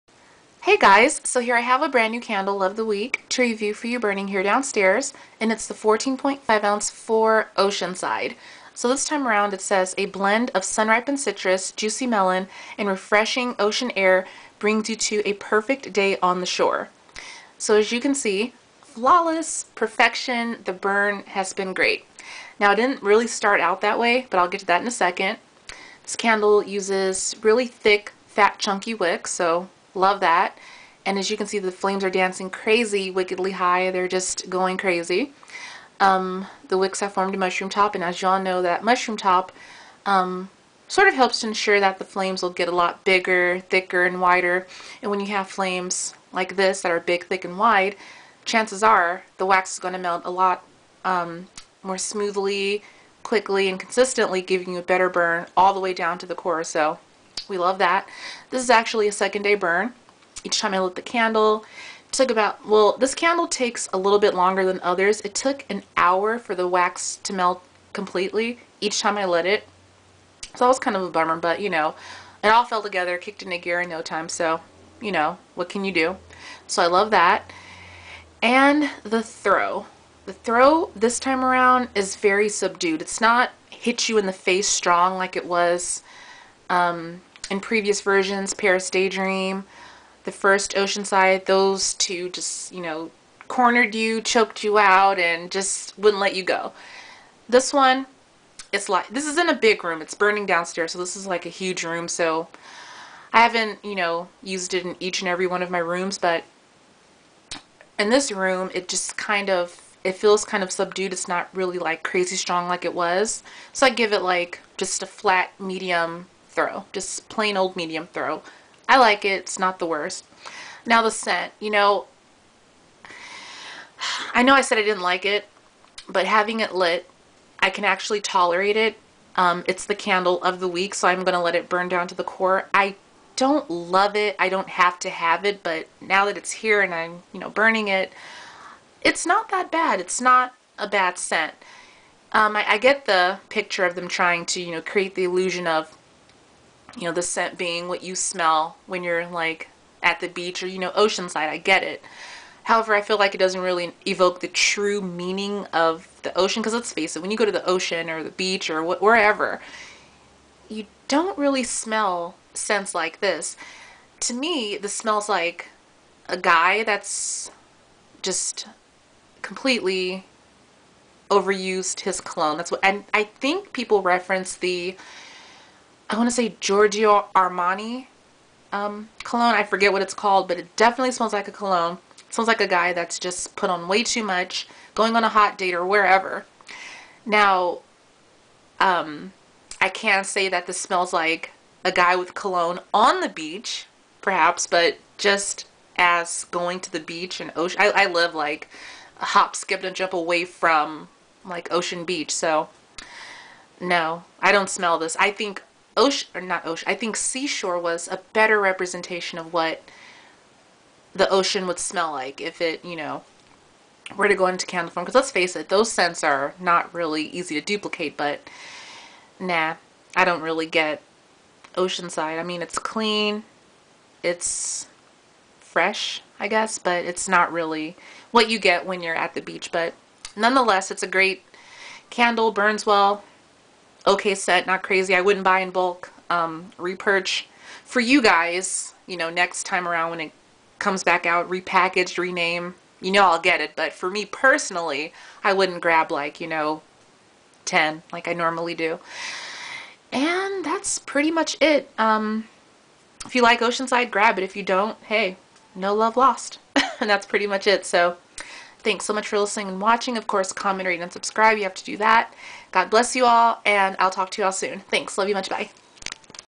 Bath and Body Works Candle Review- Candle of the Week: Oceanside (2013)
Now, I don't LOVE this scent, but I definitely don't hate it! It's tolerable! I don't feel like this is something you'd smell when you go to a beach. This smells just like men's cologne! I forgot the exact reference that people use, but it's a cologne scent. The more I burn it, the more I can tolerate and actually "enjoy" it! It smells like a guy that overused cologne. It smells like a guy on the beach with cologne! haha! The listed note aren't really present. There is a bergamot scent, but it's very subdued. This time around, "Oceanside" has a medium throw! It didn't corner me like it did while it was named "Paris Daydream" or even "Sparkling Icicles!" The burn did take some time! 1 hour! It fell together so no problems! Excellent burn! Not a repurch for me, but if you love the scent, IT'S ALL YOURS!